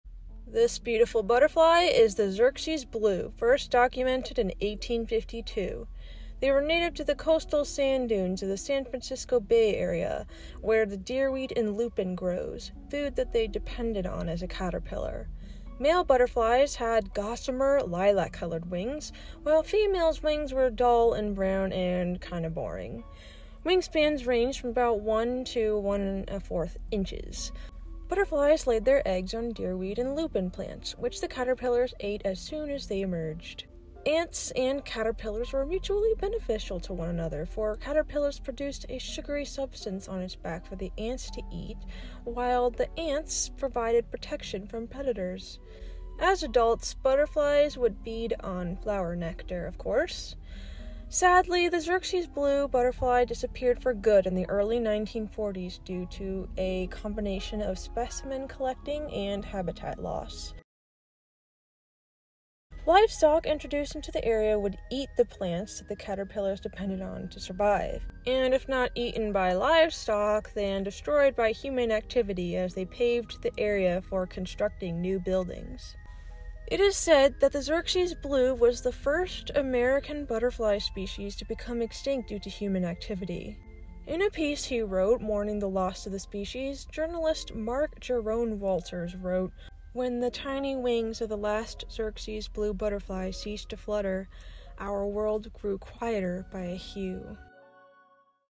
Xerces Blue Butterfly - Recently Extinct Animals
Today we remember this gorgeous butterfly called the Xerces blue : the first North American butterfly to go extinct thanks to human activity.

Sweet creature merch!!!